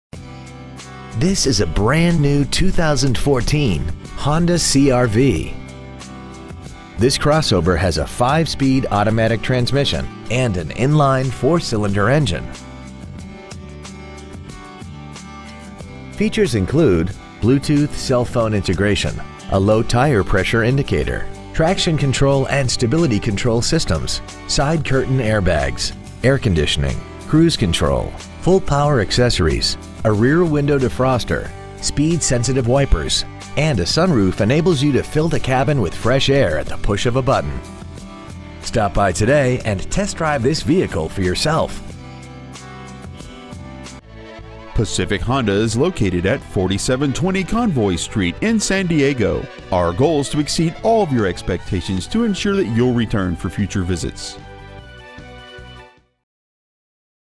2014 Honda CR-V San Diego CA
We have been honored to serve the San Diego CA area , we promise that your experience at our dealership will exceed your expectations ! Year : 2014 Make : Honda Model : CR-V Engine : 2.4L 4 cyls Trans . : Automatic 5-Speed Exterior : White Miles : 216 Stock : 141327 Pacific Honda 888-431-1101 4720 Convoy Street San Diego , CA 92111